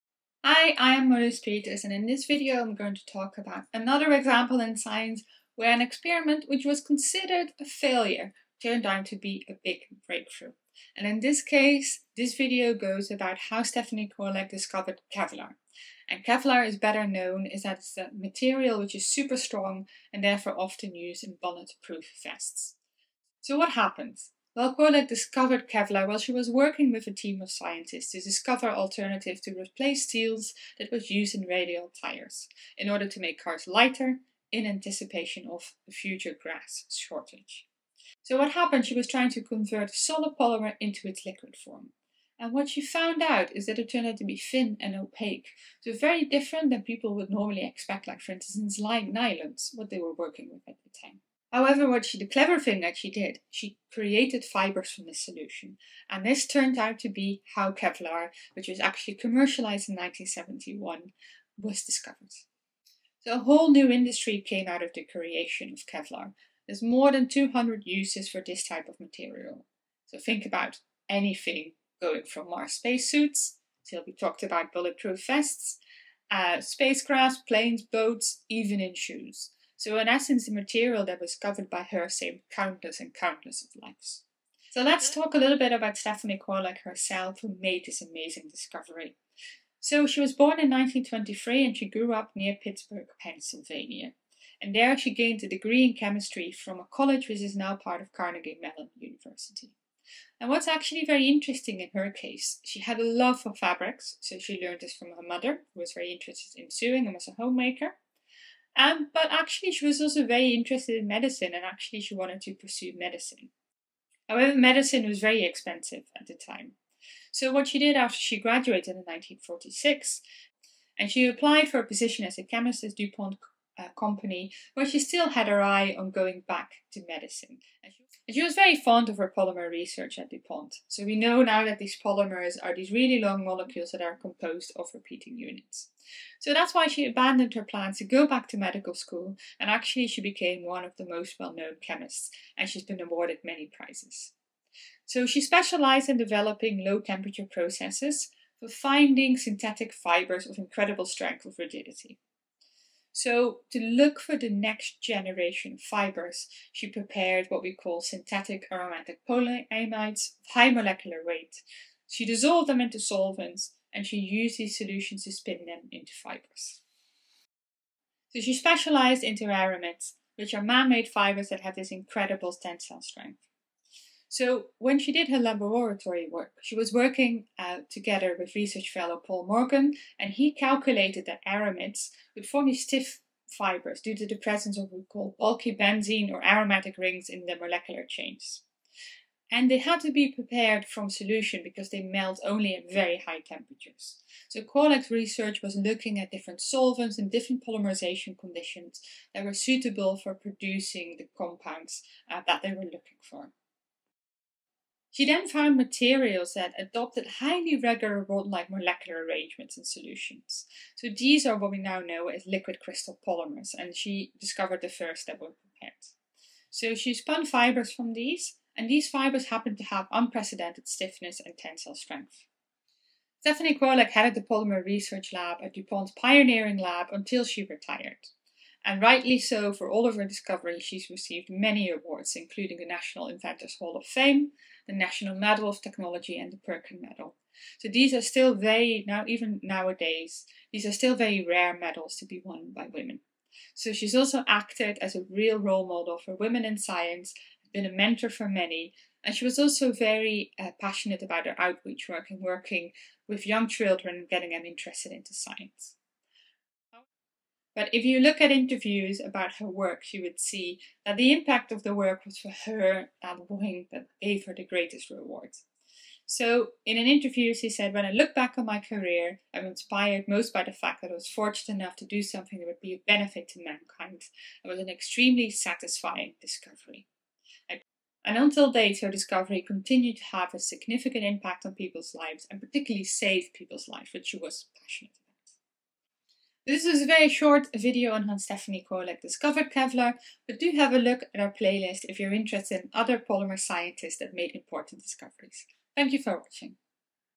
Stephanie Kwolek: the discovery of Kevlar
Stephanie Kwolek discovered Kevlar by accident: she  was trying to convert a solid polymer into a liquid form but her creation turned out thin and opaque rather than the syrupy mixture she expected. Rather than considering this experiment a failure, she spun fibres out of the materials that were so strong, not even  steel bullets could penetrate them. And this is the history of how the material, that was commercialised under the trade name Kevlar in 1971, was discovered. Kevlar  is widely used for lightweight bulletproof vests and other high-stength application and structural parts.

Stephanie Kwolek was born in 1923 and grew up near Pittsburgh, Pennsylvania, where she gained a degree in chemistry from a college that is now part of Carnegie Mellon University. She had an interest in both fabrics and medicine, but studied chemistry instead.  After graduating in 1946 with a degree in chemistry, Kwolek applied for a position as a chemist with the DuPont Company, though she still had her eye on medical school. After becoming fascinated by her research on polymers, she decided that chemistry was her passion and eventually stayed at DuPont for over 40 years. She specialized in developing low-temperature processes for finding petroleum-based synthetic fibers of incredible strength and rigidity.

Stephanie Kwolek headed polymer research at DuPont's Pioneering Lab until her retirement in 1986. She has received many awards for her achievements, including induction into the National Inventors Hall of Fame in 1994, the National Medal of Technology (1999), and the Perkin Medal – all rare honors for women. However,  she considered the greatest reward of her work the impact it had in saving people's lives.